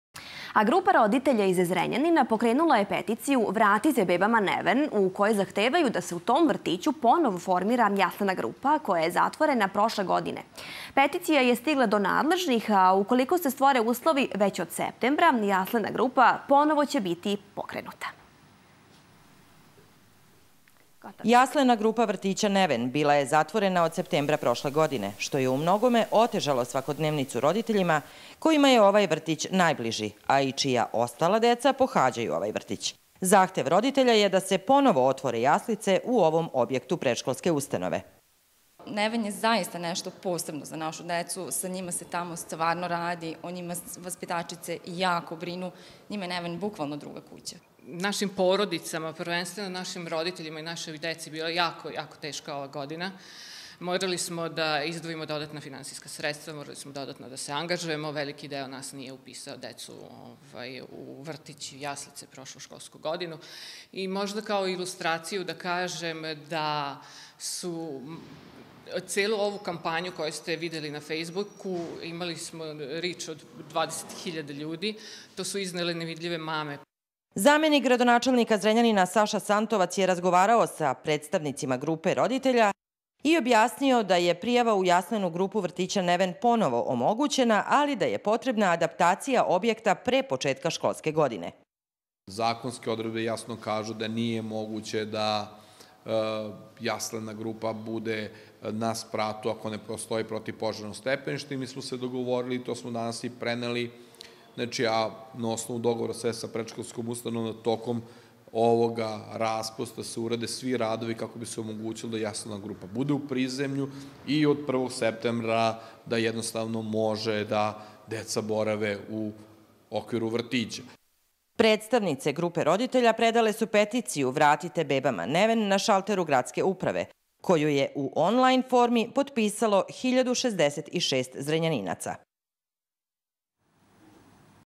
Zrenjanin: Roditelji traže da se vrtiću "Neven" vrati jaslena grupa
Grupa roditelja iz Zrenjanina pokrenula je peticiju "Vratite bebama Neven" u kojoj zahtevaju da se u ovom vrtiću ponovo formira jaslena grupa, koja je zatvorena prošle školske godine.

Peticija je stigla do nadležnih, a ukoliko se stvore uslovi već od septembra jaslena grupa ponovo će biti pokrenuta.